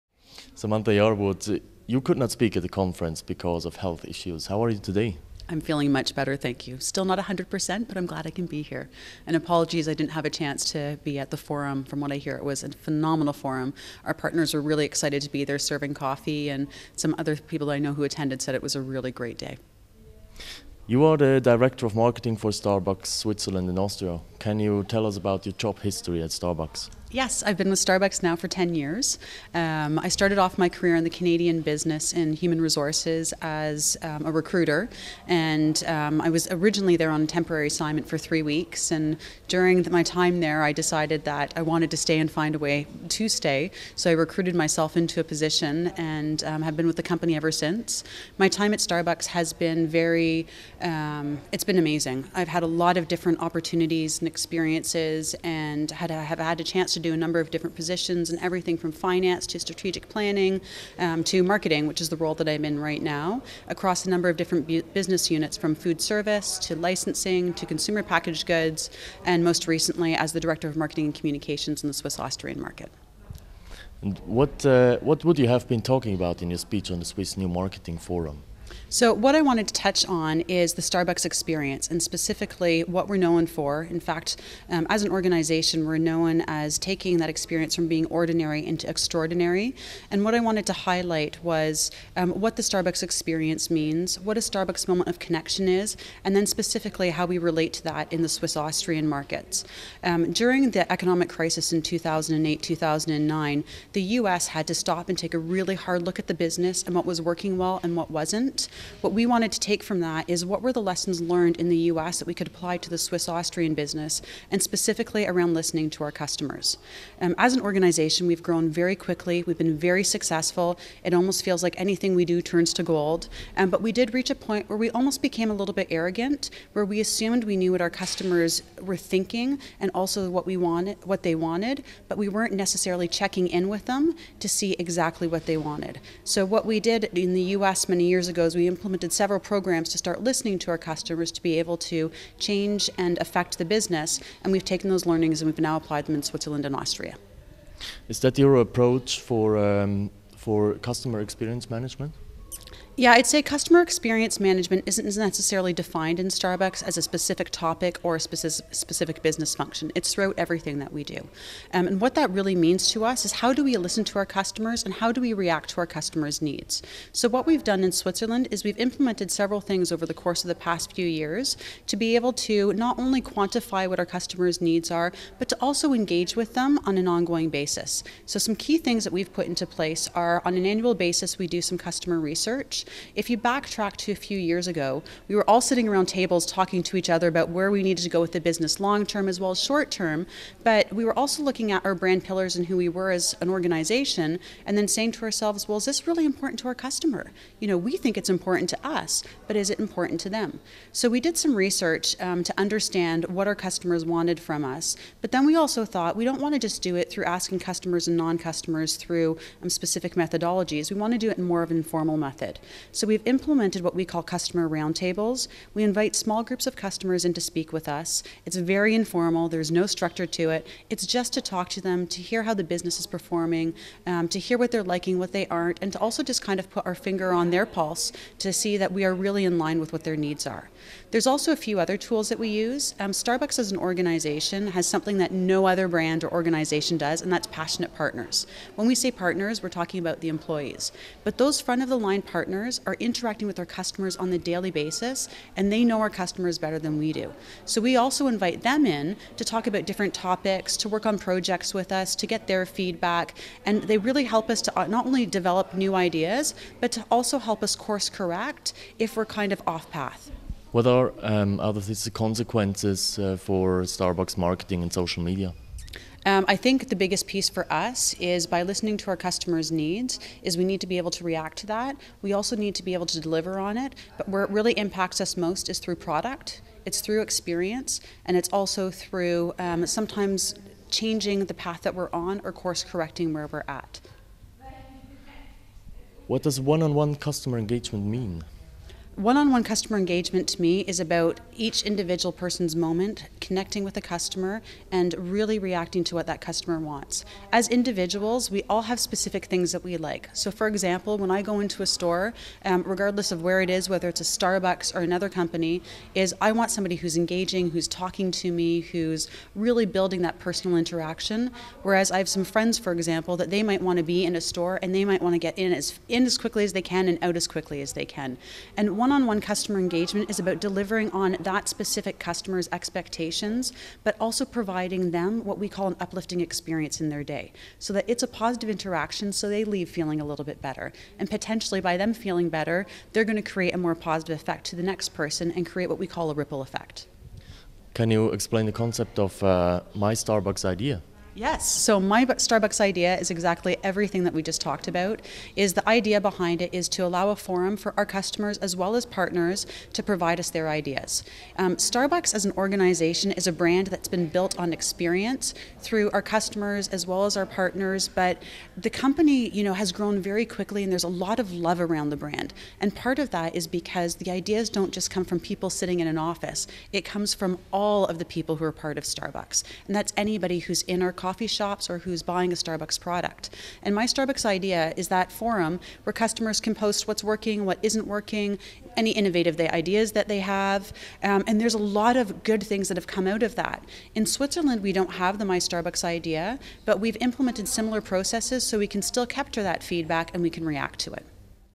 Interview mit Samantha Yarwood
Interview mit Samantha Yarwood, Director Marketing & Communication von Starbucks CH & AT, im Nachgang zum Swiss New Marketing Forum vom 12. September 2012 an der ZHAW School of Management and Law in Winterthur.